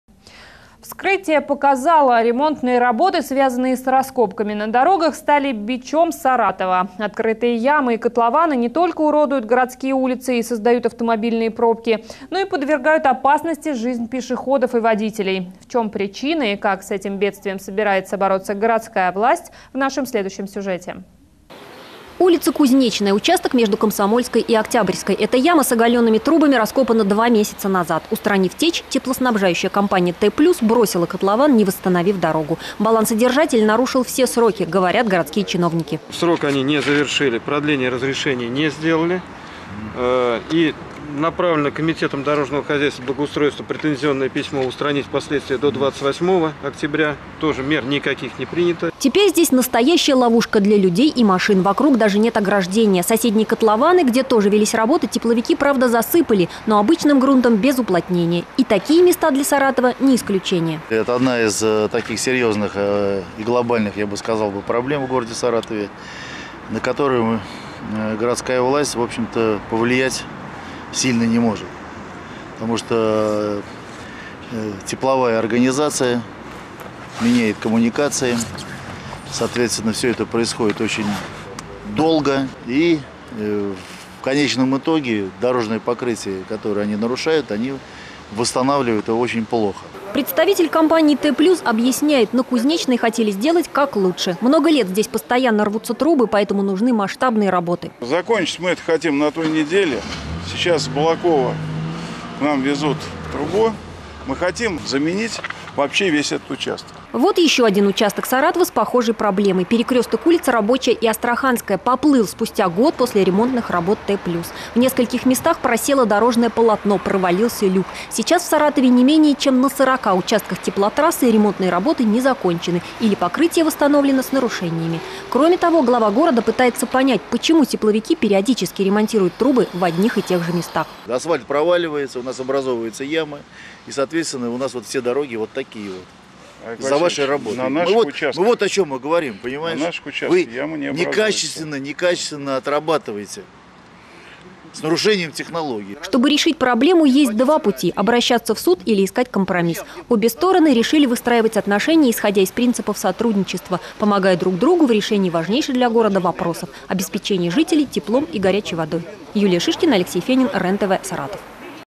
Ремонт дорог после замены коммуникаций стал бичом Саратова
Некачественно восстановленный асфальт, ямы и котлованы не только уродуют городские улицы и создают автомобильные пробки, но и опасны для пешеходов и
водителей. В чем причина и как с этим бедствием борется городская власть, в нашем сюжете от 3 ноября 2015 года.